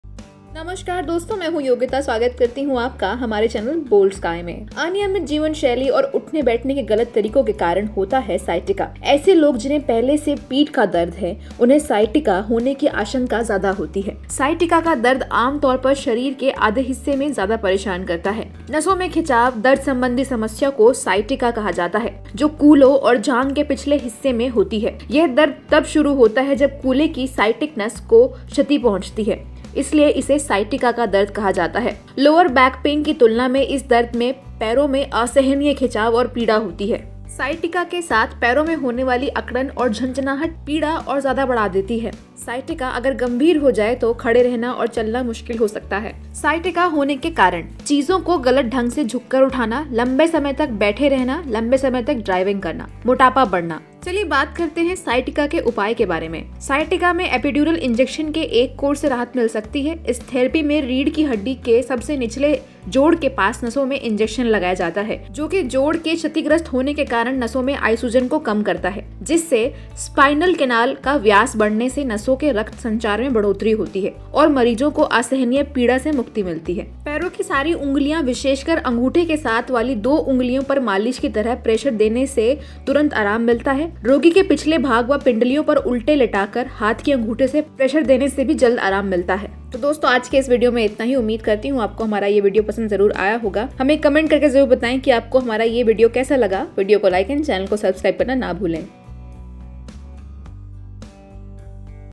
Thigh Pain होना Sciatica के Symptoms, क्या है कारण और उपाय |Boldsky
अनियमित जीवनशैली और उठने-बैठने के गलत तरीकों के कारण होता है साइटिका। ऐसे लोग जिन्हें पहले से पीठ का दर्द है, उन्हें साइटिका (शियाटिका) होने की आशंका ज्यादा होती है। साइटिका का दर्द आमतौर पर शरीर के आधे हिस्से में ज्यादा परेशान करता है।

Sciatica is caused due to irregular lifestyle and wrong way of sitting. People who already have back pain are more prone to sciatica. The pain of sciatica usually bothers more in one half of the body.